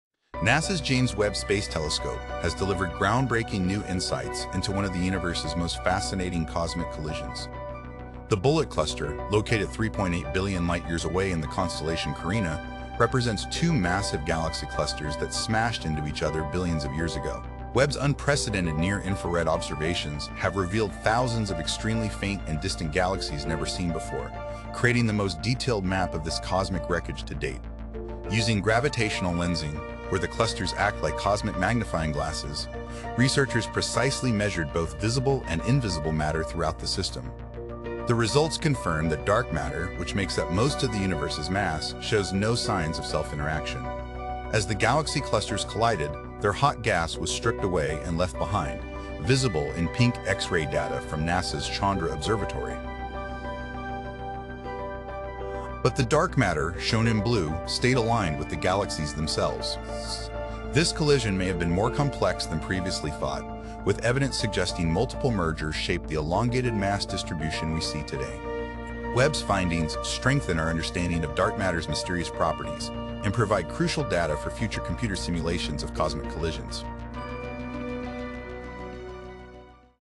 NASA Webb Telescope Reveals Secrets of Bullet Cluster
The powerful James Webb Space Telescope has peered deep into the Bullet Cluster, uncovering new insights about this iconic cosmic collision.